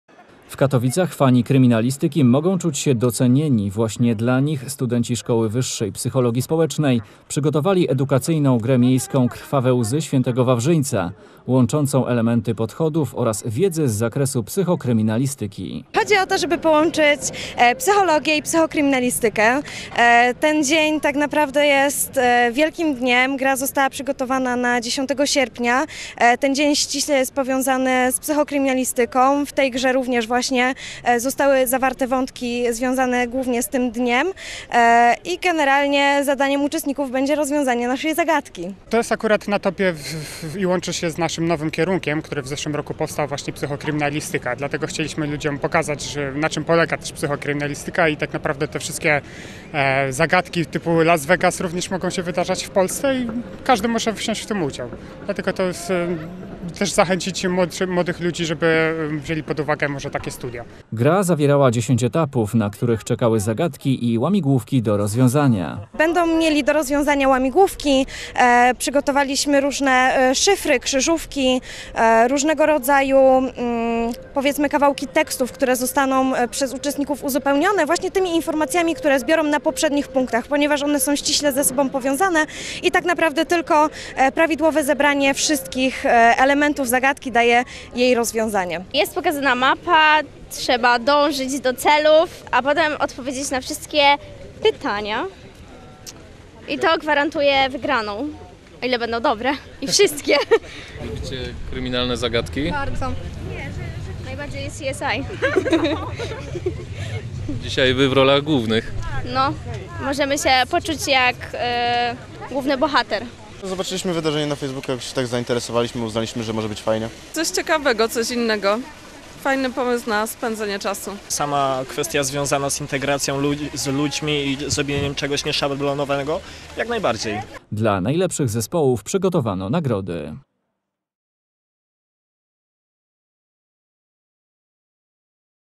KRYMINALNE ZAGADKI KATOWIC
W Katowicach fani kryminalistyki mogą czuć się docenieni. Właśnie dla nich studenci Szkoły Wyższej Psychologii Społecznej przygotowali edukacyjną grę miejską "Krwawe łzy św. Wawrzyńca", łączącą elementy podchodów oraz wiedzy z zakresu psycho-kryminalistyki.